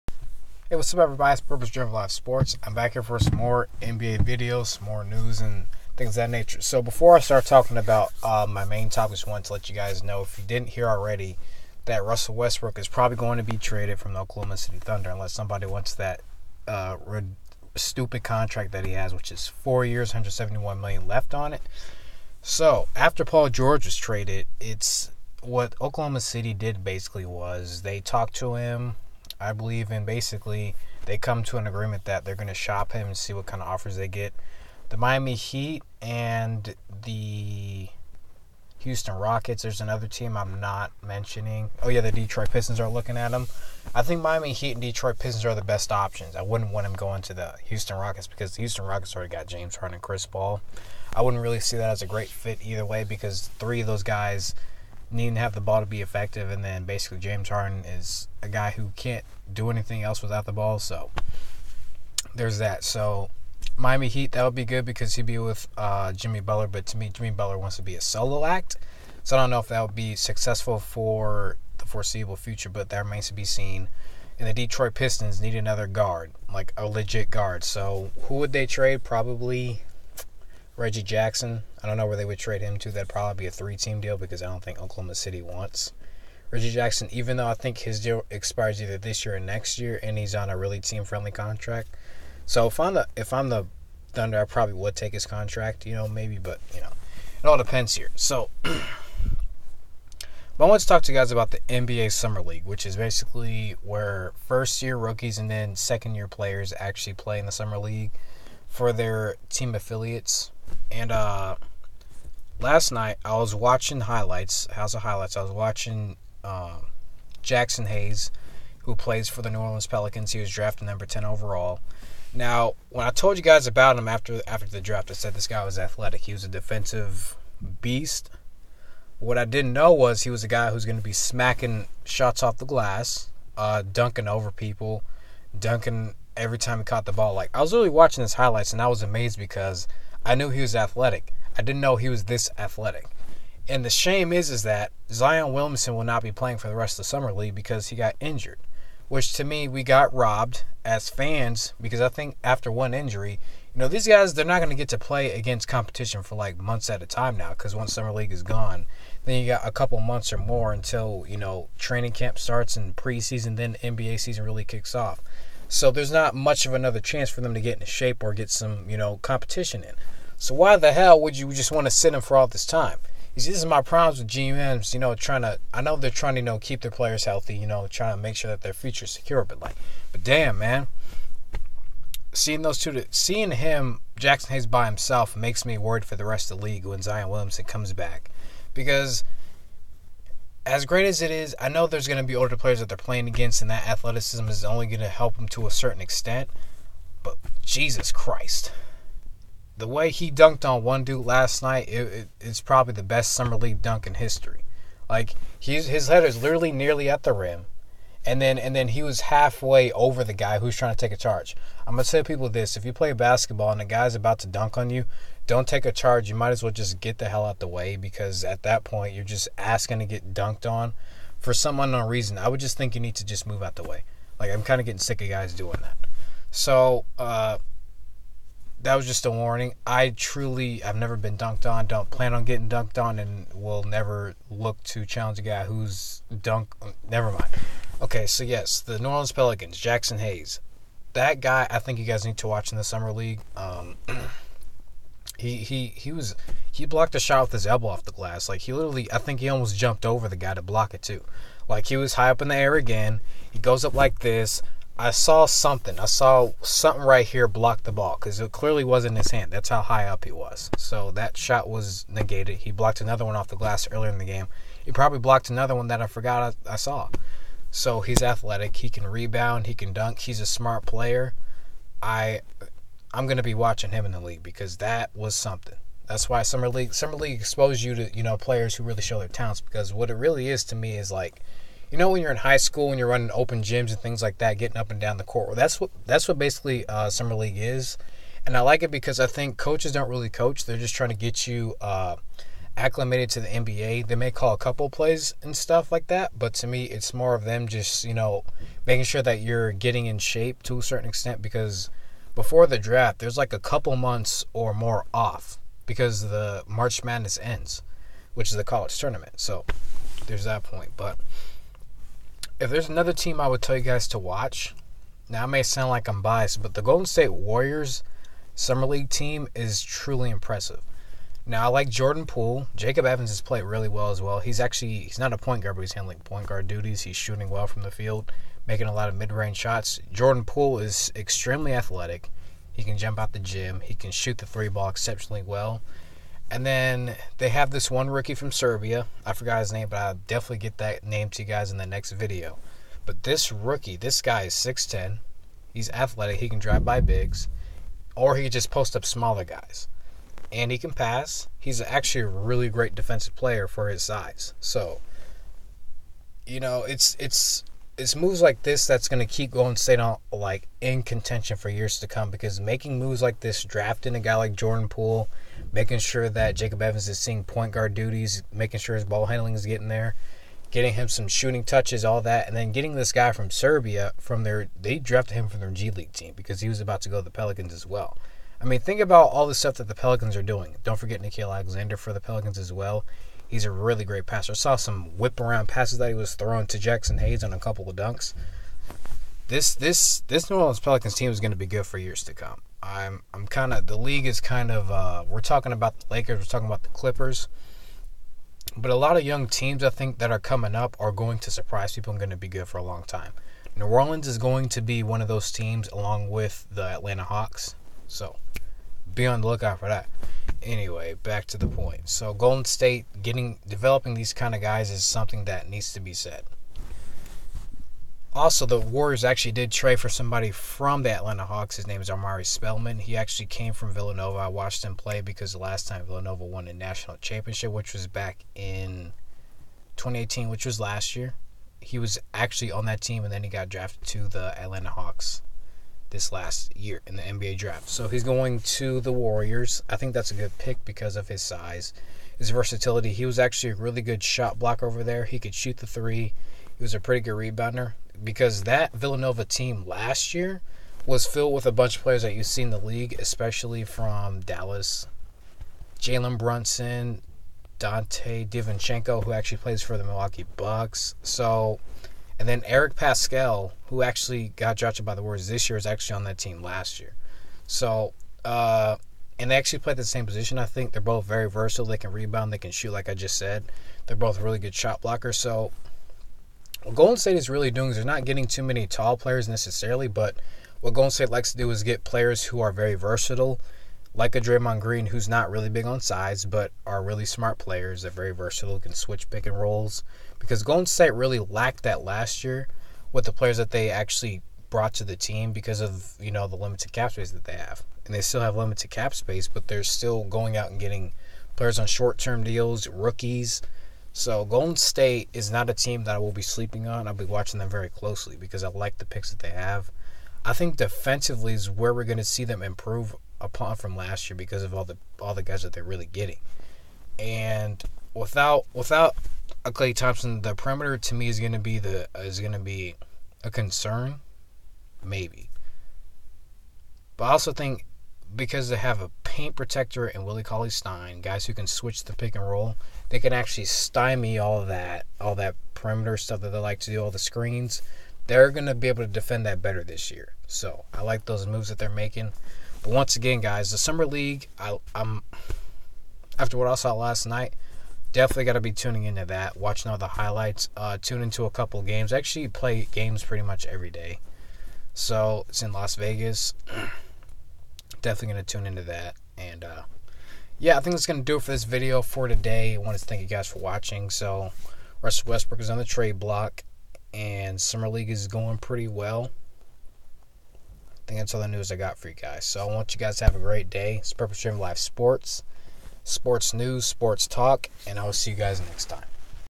NBA Summer League Shows Potential Of The Young Players
In this video, I talk about the NBA Summer League and its impact. Young NBA players in this tournament get to really showcase their ability and show us their potential in a cool environment, almost related to a high school open gym. I also let you guys know Russell Westbrook is on the trade block.